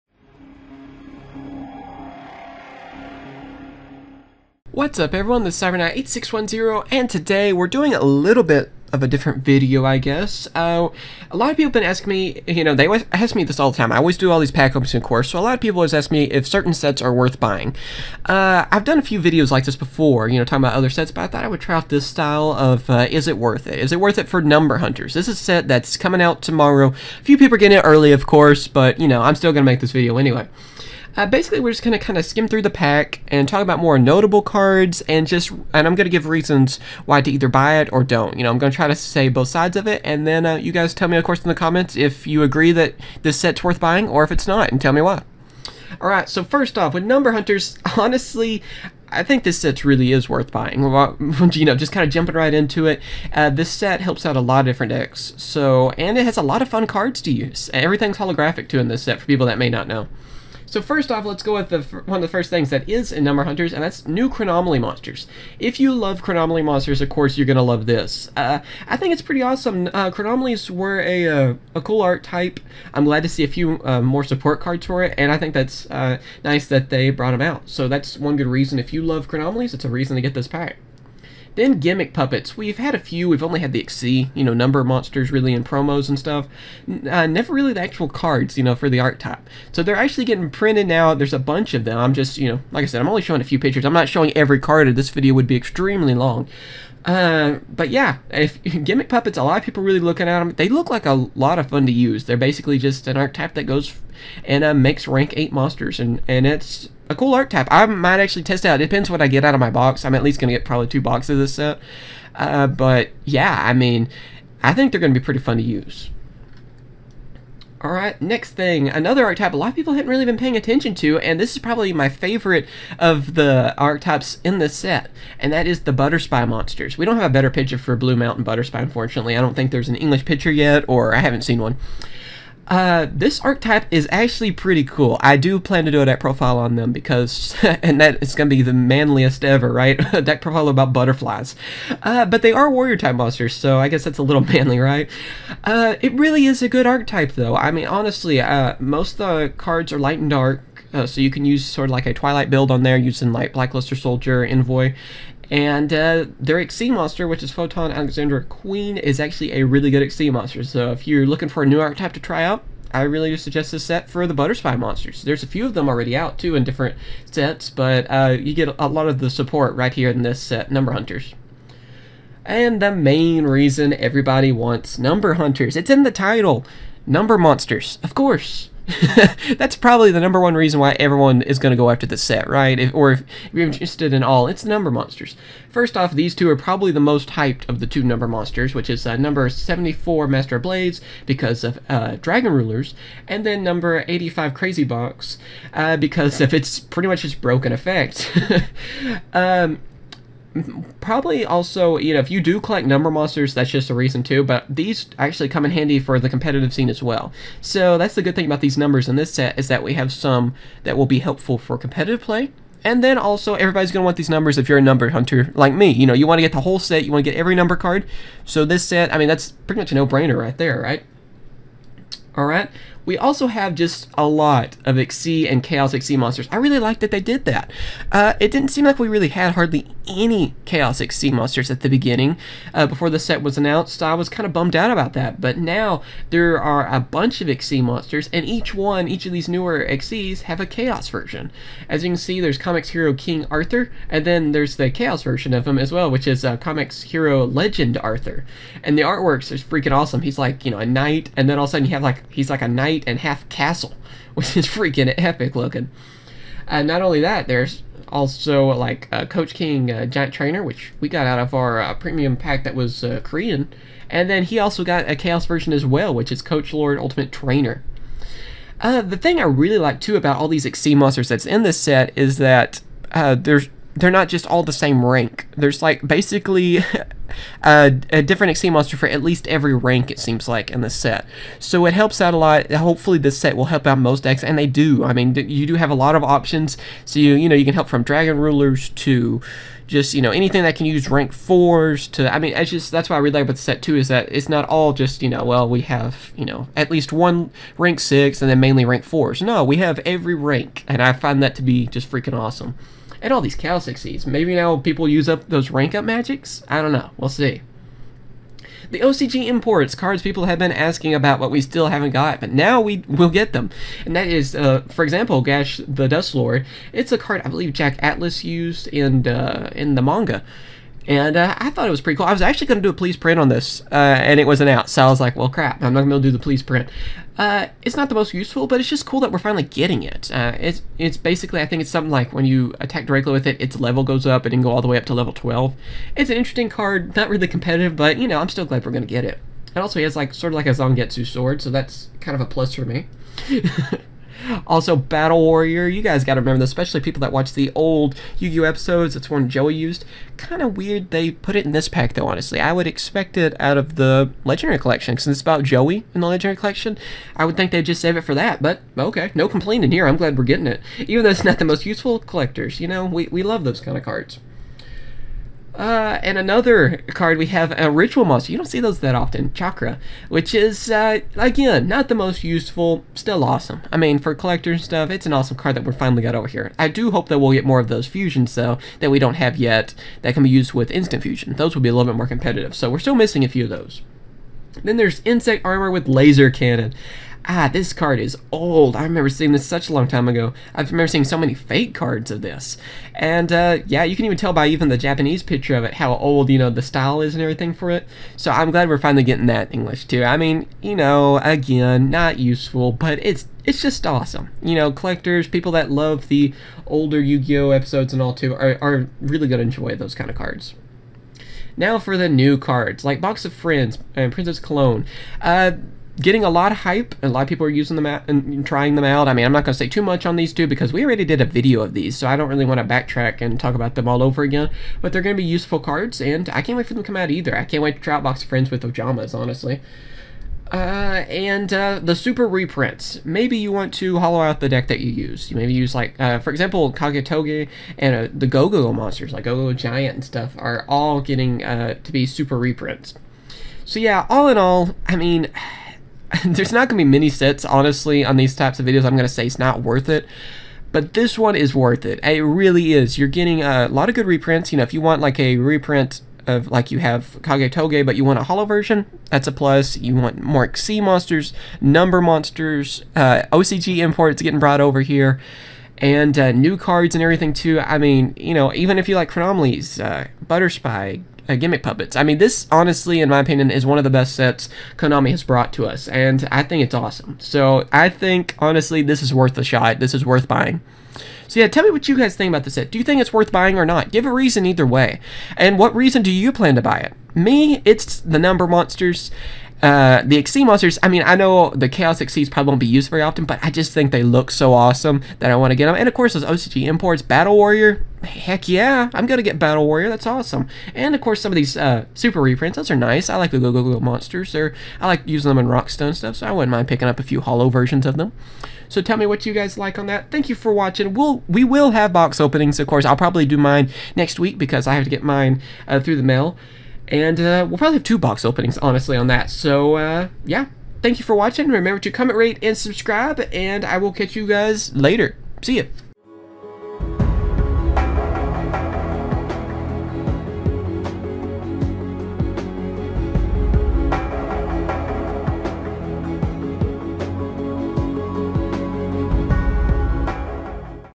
Yugioh Number Hunters Worth Buying?
Sort of a late video seeing is that some people are already getting this pack but I wanted to get the full card list before making this video. I think this video will give new players a good idea of this set. Will you all buy this set? I hope you all enjoy!